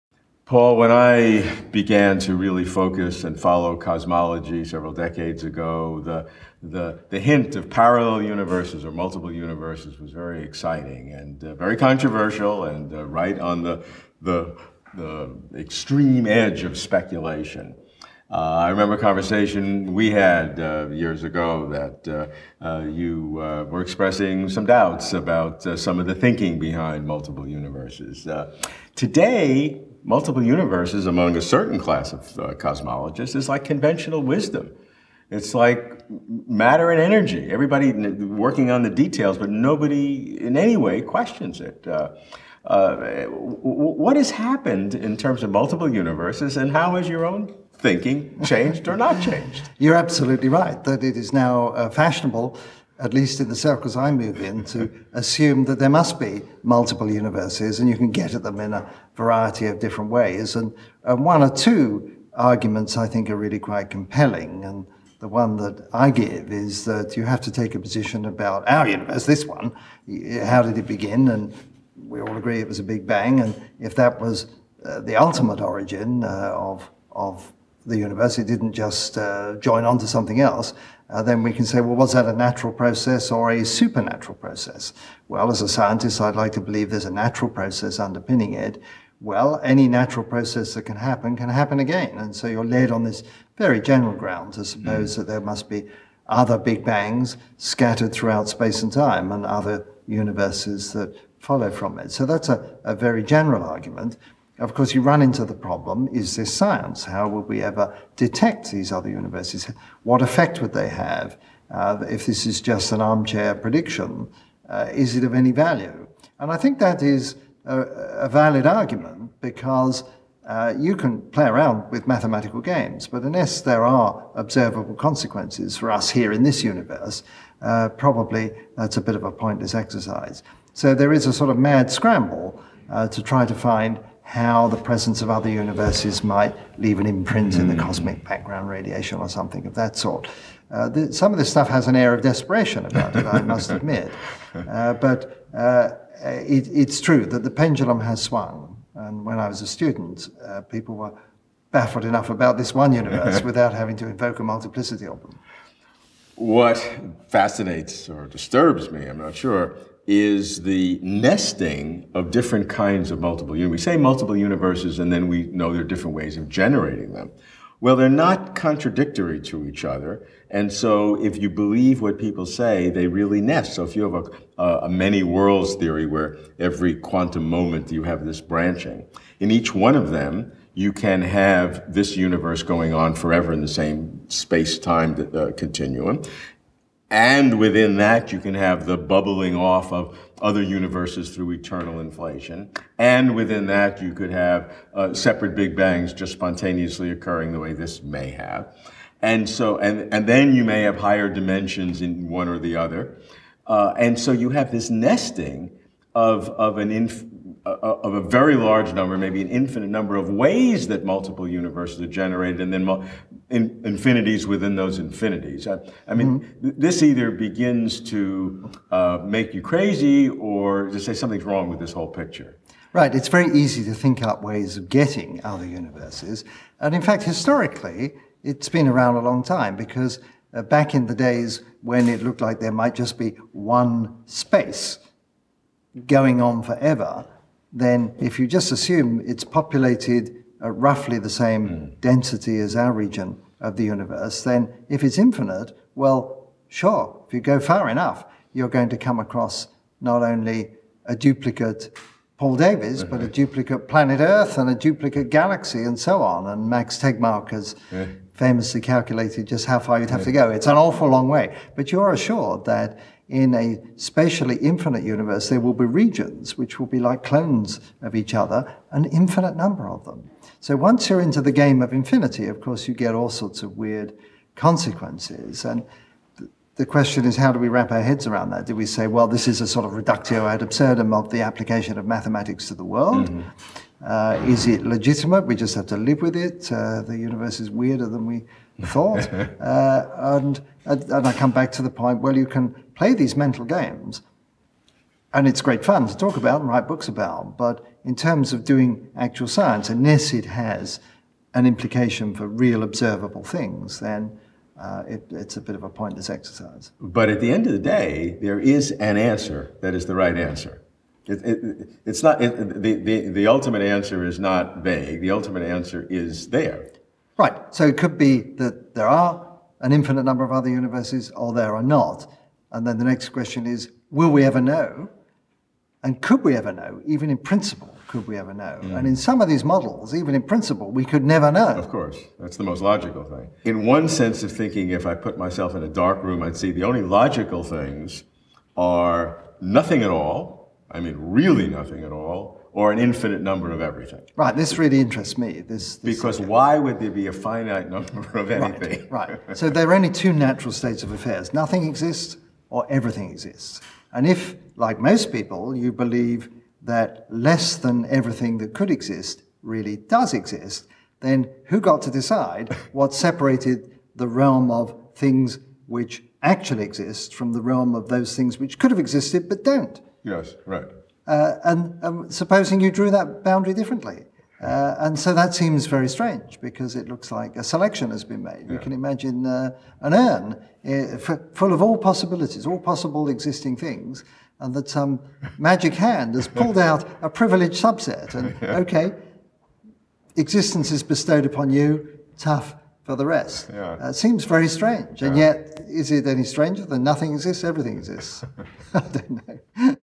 Paul Davies - Do Multiple Universes Surely Exist?
Multiple universes? How can scientists come to believe in more than one universe? The idea sounds astounding. Our universe is immense by itself. How could there be more than one? What's more, multiple universes can be generated by radically different mechanisms. But what's a 'universe' anyway? The whole vast ensemble of universes is now called a 'multiverse'.

Paul Davies is a theoretical physicist, cosmologist and astrobiologist.